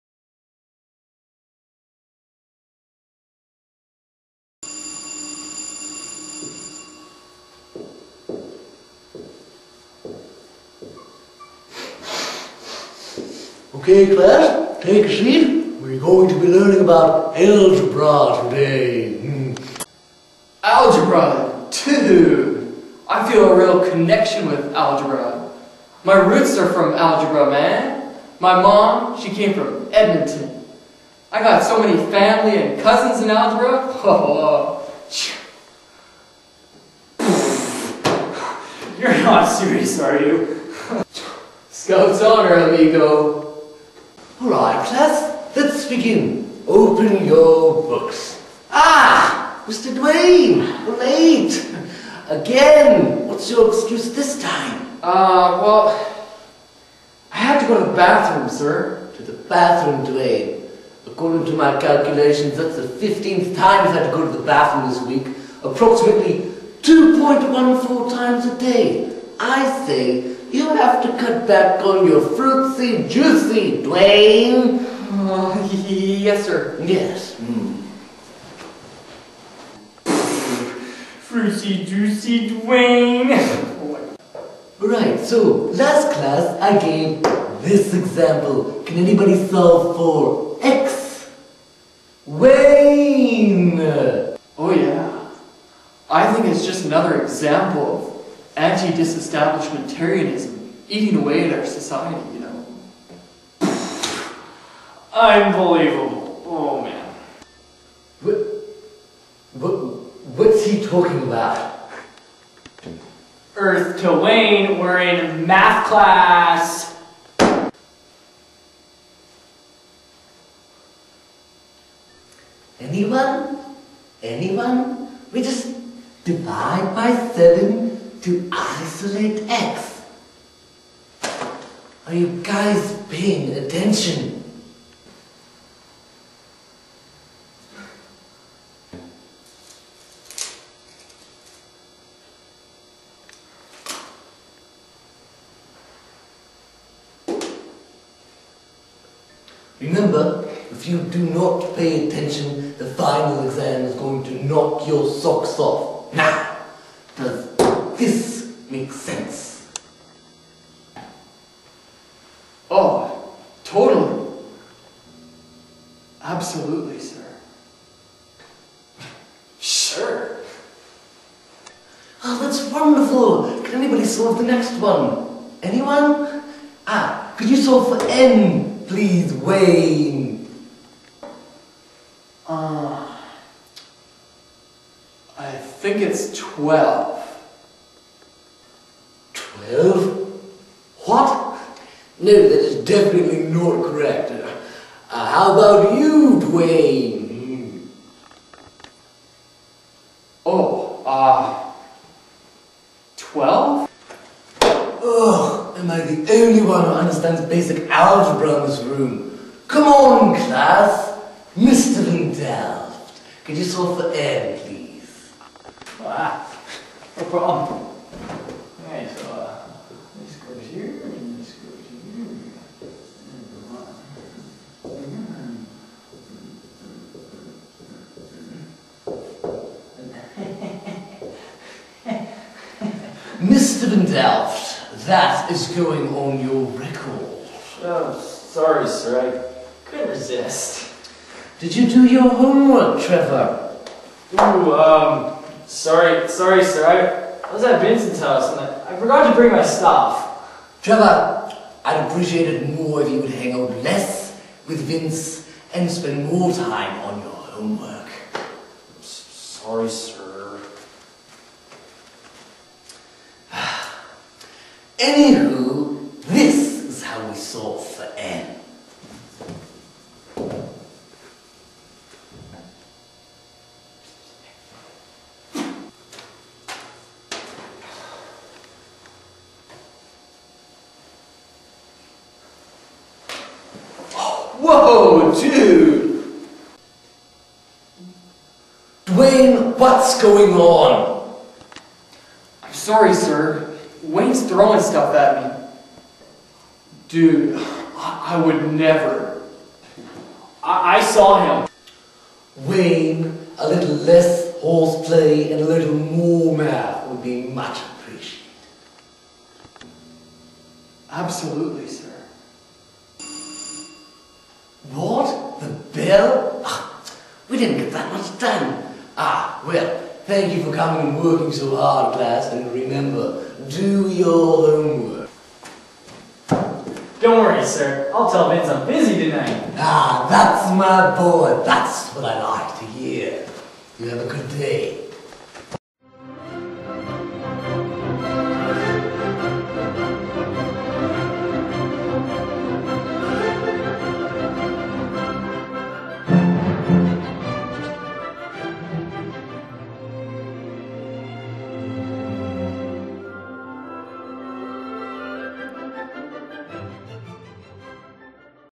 Algebra 101
Dwayne, Wayne, and Trevor learn algebra.